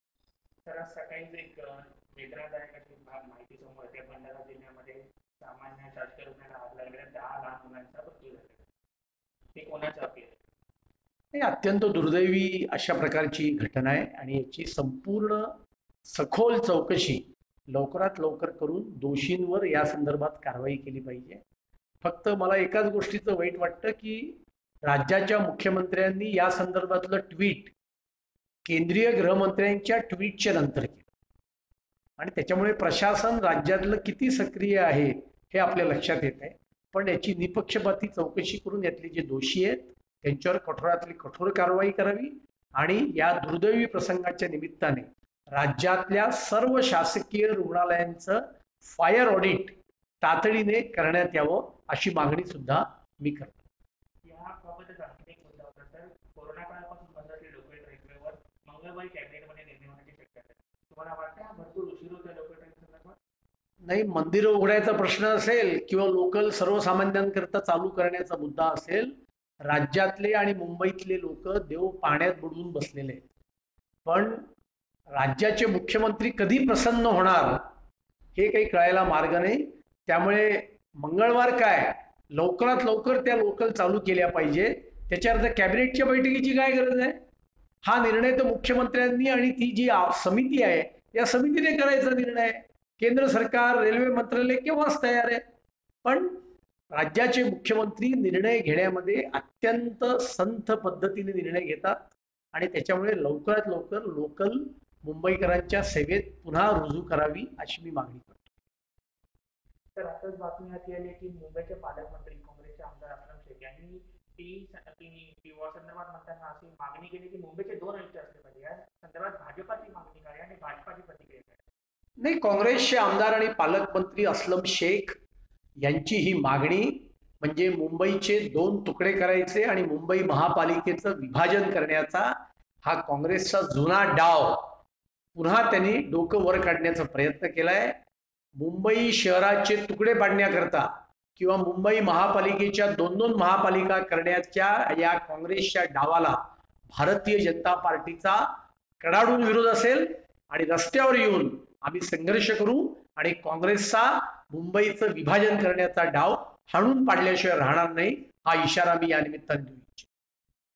मुख्यमंत्र्यांआधी अमित शहांनी ट्विट केलं-अतुल भातखळकर
भंडारा-भंडारा जिल्हा शासकीय रुग्णालयाच्या अतिदक्षता नवजात शिशु केअर युनिटला शनिवारी मध्यरात्रीच्या सुमारास आग लागली आहे. लागलेल्या आगीत 10 बाळांना मृत्यू झाला आहे. मध्यरात्री २ च्या सुमारास शॉर्ट सर्किटमुळे ही आग लागल्याचं सांगितलं जात आहे. धुरामुळे गुदमरुन या बाळांना मृत्यू झाल्याचं बोललं जात आहे.

शनिवारी मध्यरात्रीच्या सुमारास जिल्हा शासकीय रुग्णालयातील नवजात बालकांच्या अतिदक्षता विभागात धूर निघत असल्याचं पाहायला मिळालं. तेव्हा कामावर असलेल्या स्टाफने दार उघडून पाहिलं असता आतमध्ये मोठ्या प्रमाणात धूर निघत असल्याचं तिच्या लक्षात आलं. तेव्हा याची माहिती तातडीने वरिष्ठांना देण्यात आली. सरकारी रुग्णालयातील कर्मचाऱ्यांच्या सतर्कतेमुळे ७  बाळांचा जीव वाचवण्यात यश आलं आहे.

यावर भाजप प्रवक्ते अतुल भातखळकर यांनी भंडारा जिल्हातील घटना दुर्दैवी आहे,असं म्हणत अतुल भातखळकरांनी मुख्यमंत्री उद्धव ठाकरेंवर निशाणा साधला आहे.गृहमंत्री अमित शहांनी मुख्यमंत्री उद्धव ठाकरे यांच्याआधी ट्विट केल्याने त्यांनी मुख्यमंत्र्यांवर टिकास्त्र डागलं.ebook.com/rnonewsonline/